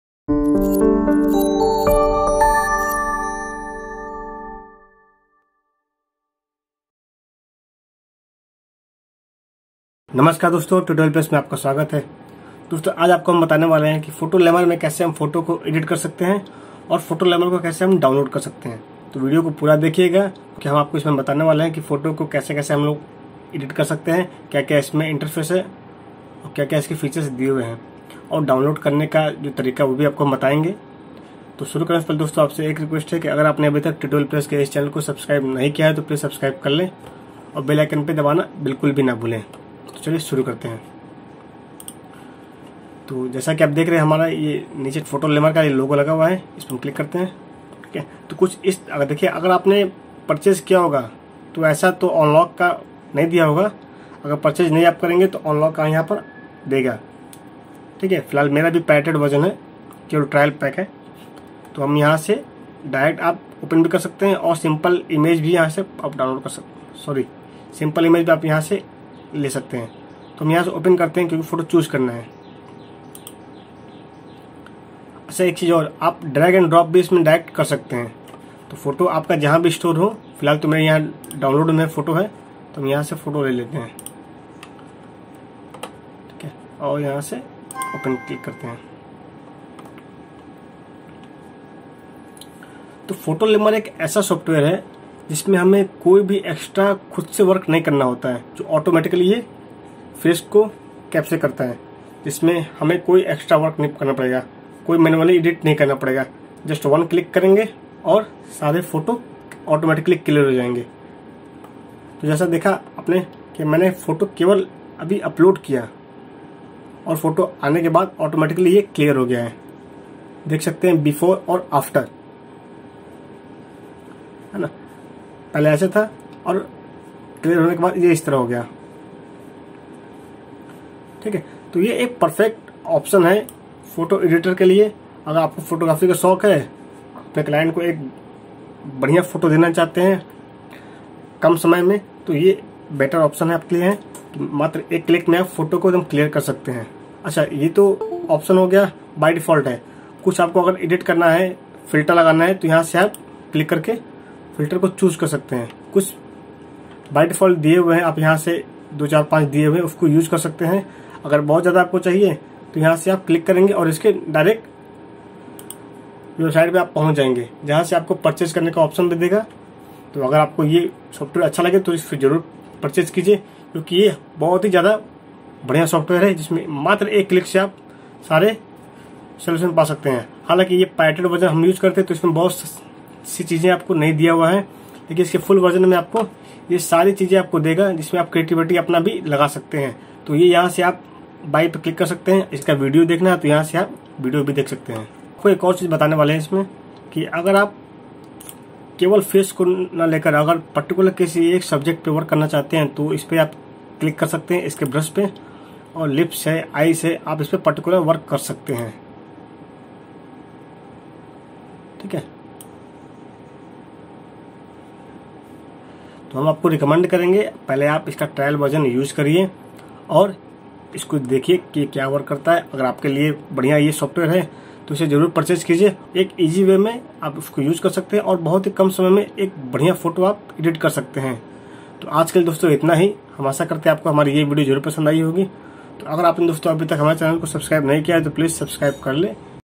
High-End Skin Softening and Retouching in 30 Second in Photolemur | Complete Photolemur Tutorial
AMAZING way to smooth skin,  in Photolemur . Using this quick high-end skin retouching technique, in 30 Second or less. Also, at the end of the video, I'll show you how to do the same thing in 20 seconds or less!